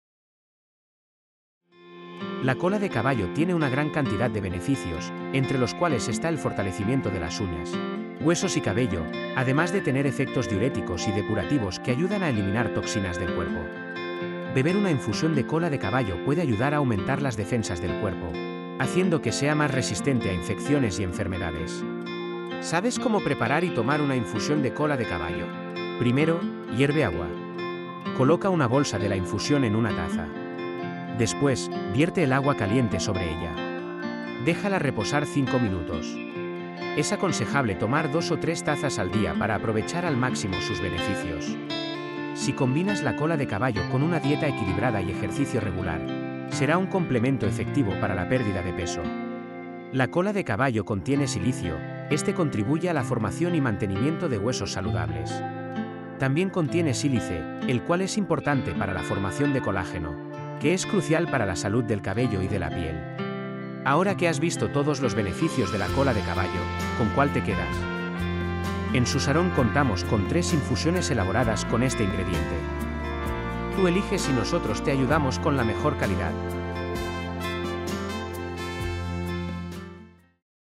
Cola de caballo, un remedio natural para tu salud
La cola de caballo ha sido utilizada durante años en la medicina natural por sus increíbles efectos en el cuerpo. En este vídeo te contamos sus numerosos beneficios.